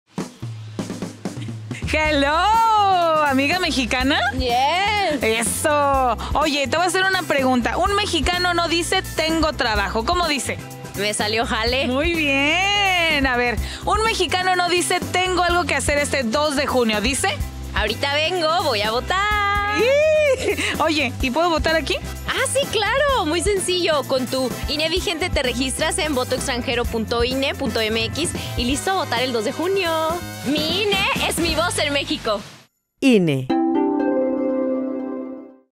Mi INE es mi voz en México. (Voto en el extranjero 2)
Si eres mexicana o mexicano y vives en el exterior, no olvides acudir a tu consulado más cercano para tramitar tu Credencial para Votar y puedas ejercer tu VotoExtranjero.